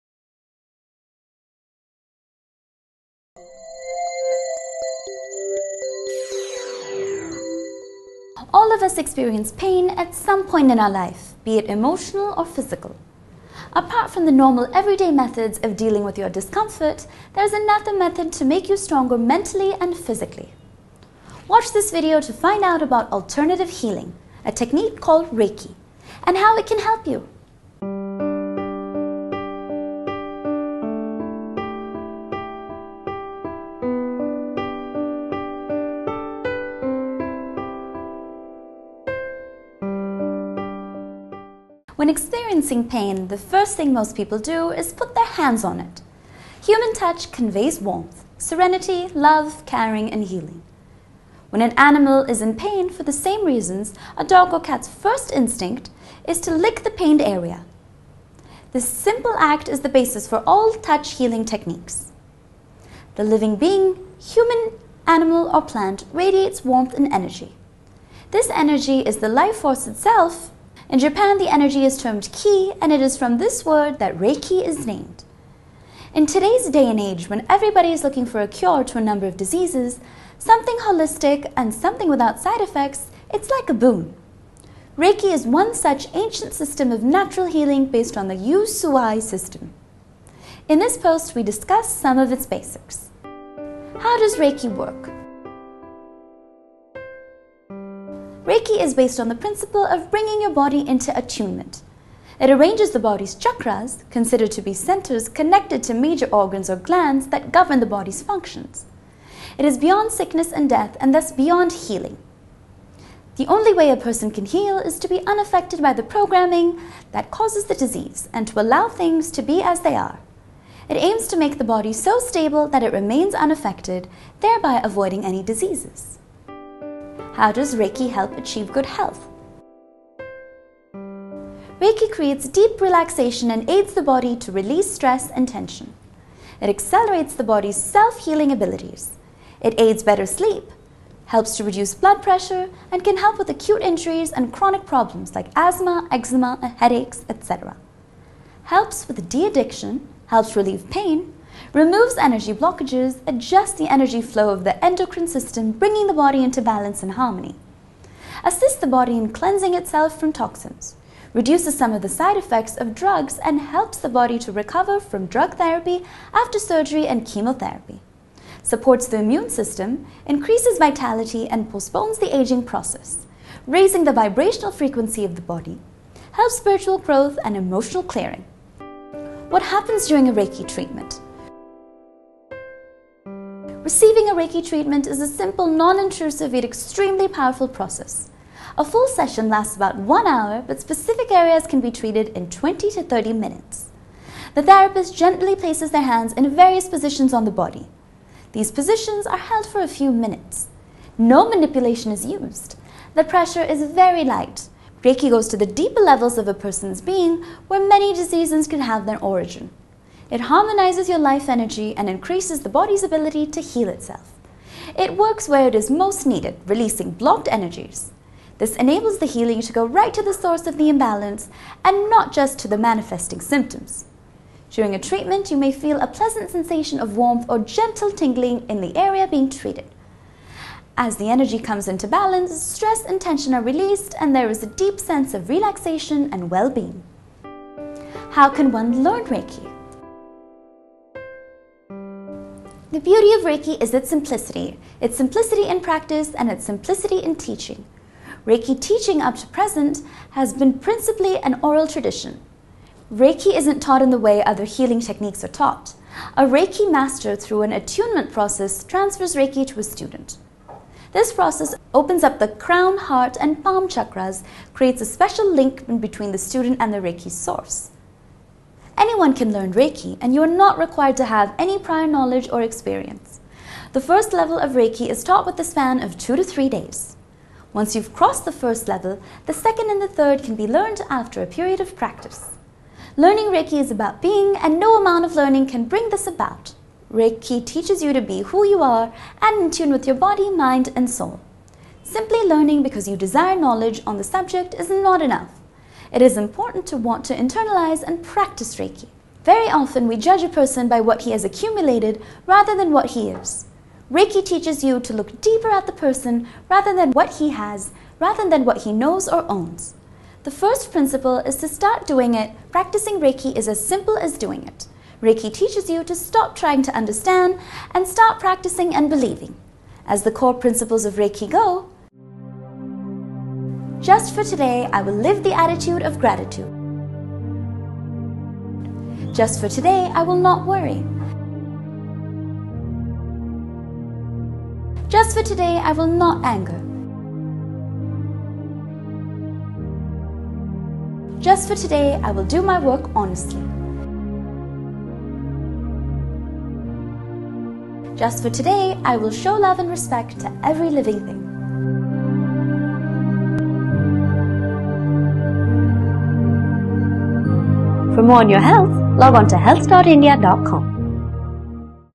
Reiki: All you need to know
Find out a unique alternative healing thechnique that can relax and recharge your batteries after that really tough day. You can also learn the science quickly and easily. Watch this video to find out more about the wonder healing technique.